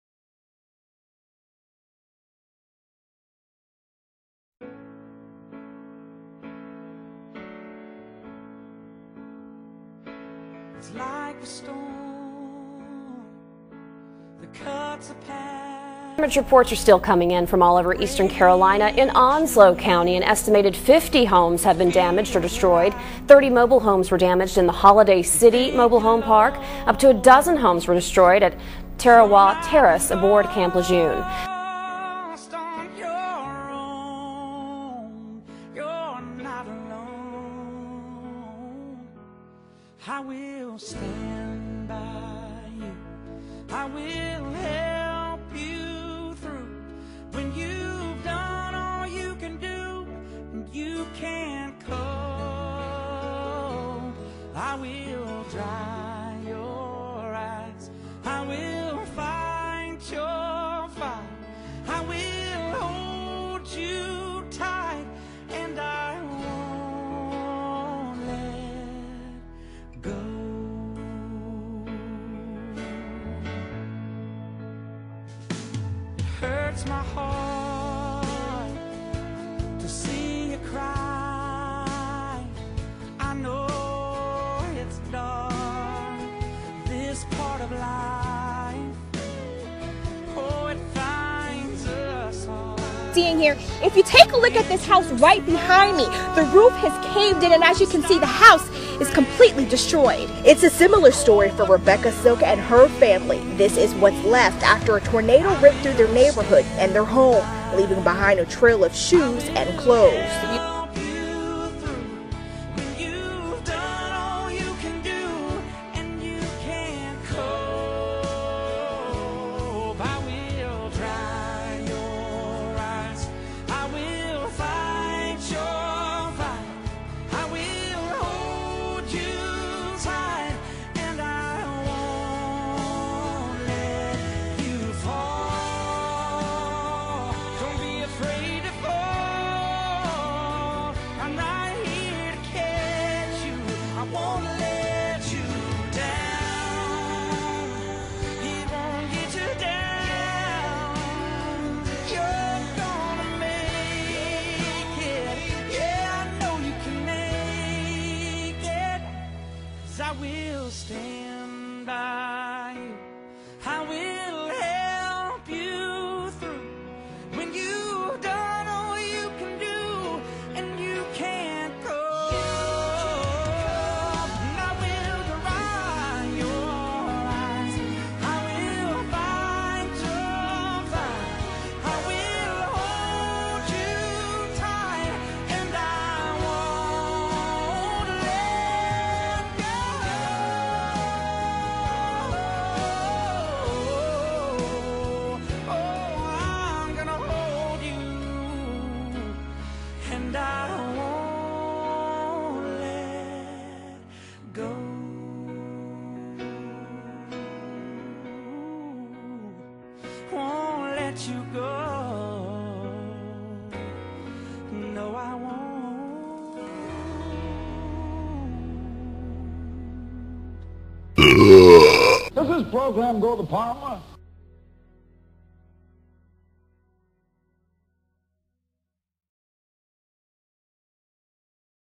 Jacksonville, NC Tornado
On April 16, 2011 there was a tornado outbreak in North Carolina. One of the cities hit hard was Jacksonville, NC at 8:30pm with 145mph winds. Chelsea this is for YOU!!